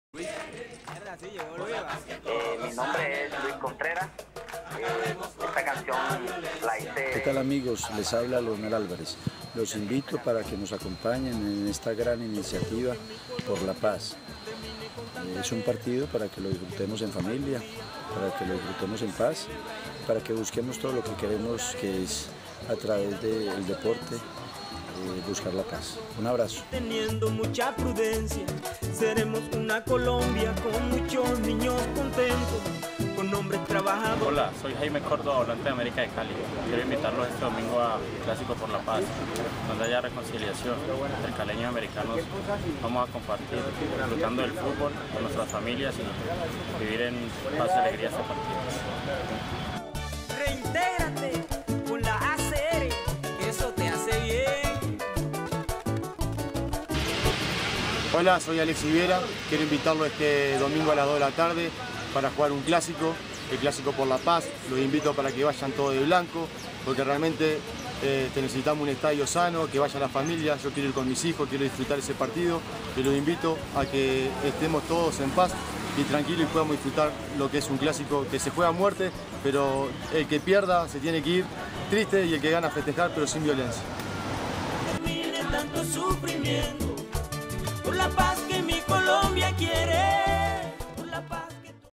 Clásico Blanco por la Reintegración, la Reconciliación y la Paz de Colombia
La tarde del domingo 20 de enero, en el Pascual Guerrero, tendrá fútbol por la paz, la reconciliación y la reintegración de personas desmovilizadas en Colombia. Personalidades del fútbol, del Gobierno Nacional y los gobiernos locales apoyan la iniciativa.